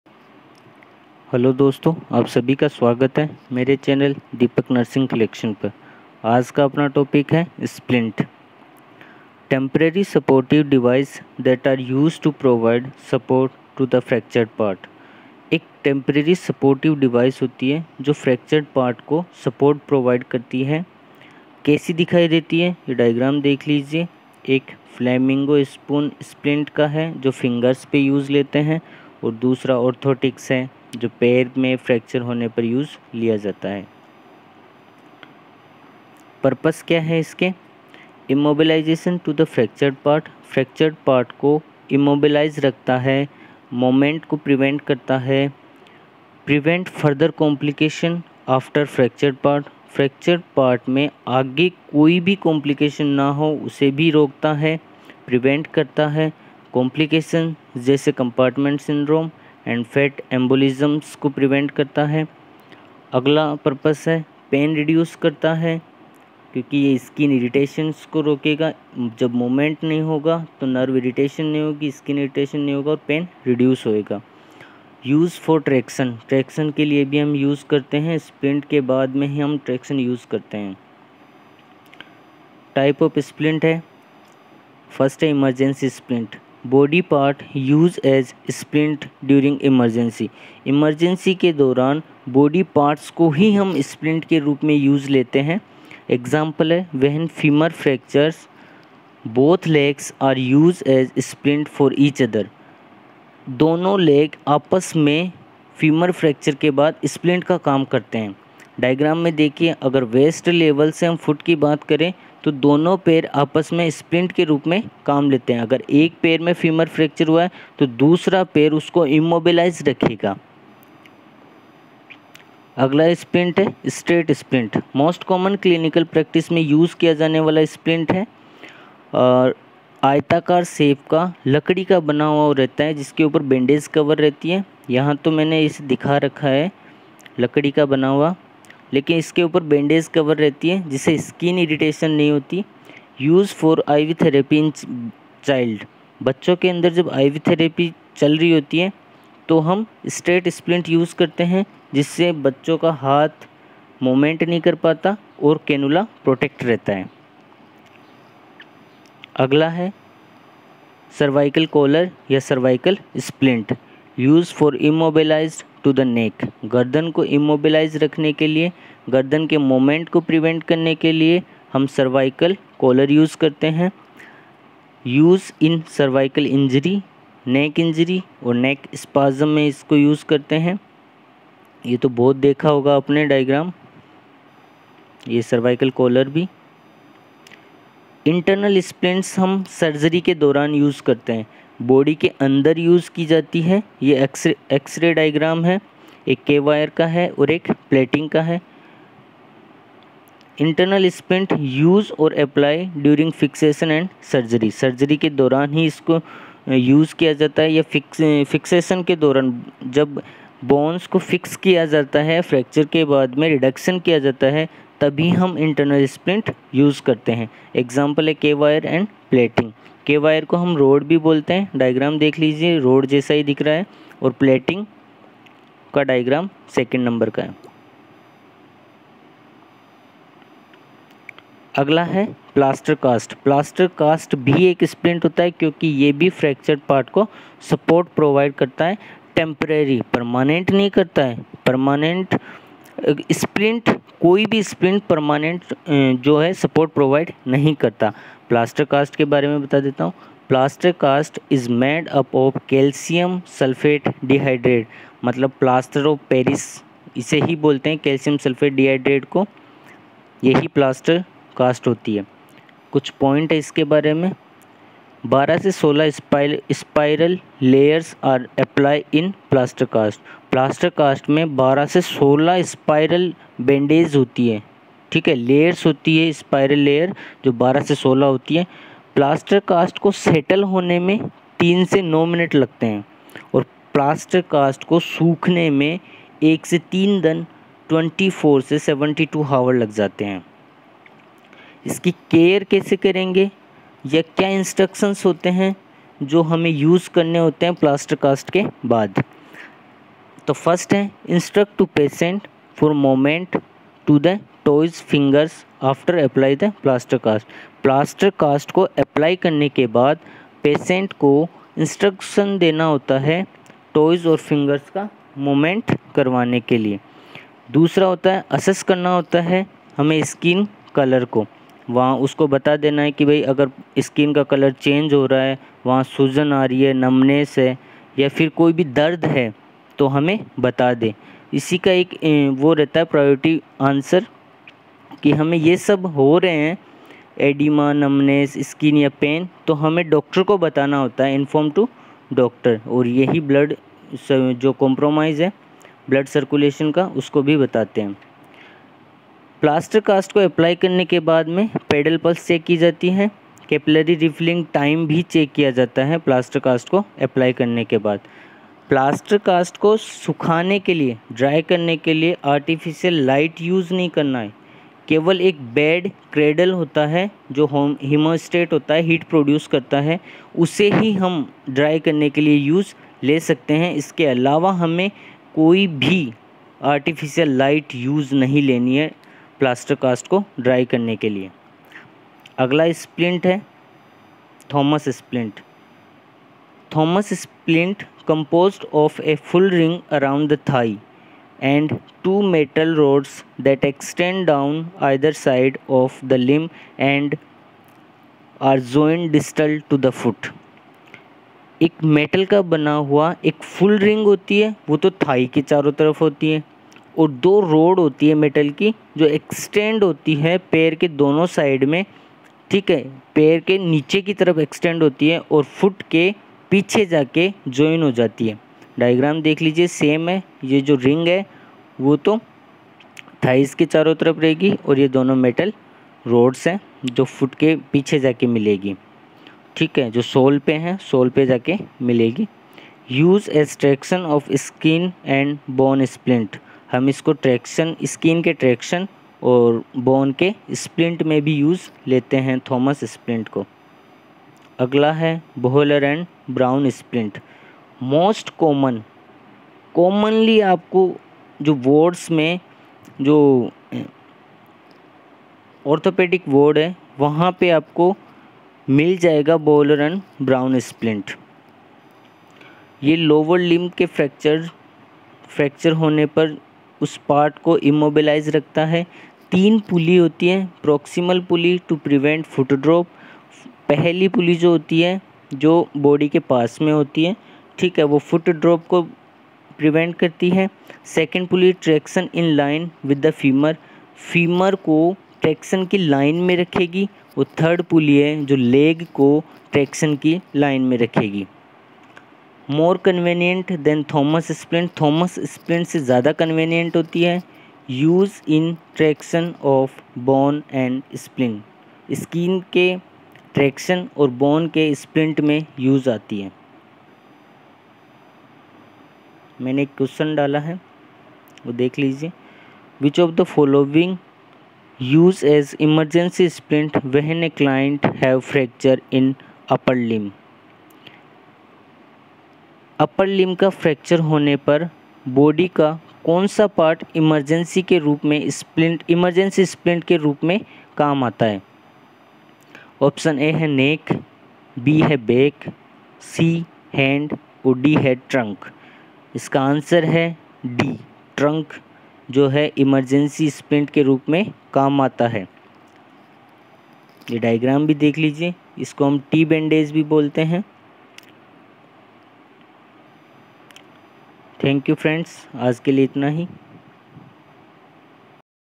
Splint with Image # fundamental of nursing # staff nurse preparation#important for AIIMS,PGI,DSSSB
Topic - Splint

Subj. -Foundation

a rigid or flexible device that maintains in position a displaced or movable part; also used to keep in place and protect an injured part” or as “a rigid or flexible material used to protect, immobilize, or restrict motion in a part.”

Important topic for all AIIMS PGI DSSSB GMCH CHO etc.

Previous video on Restrain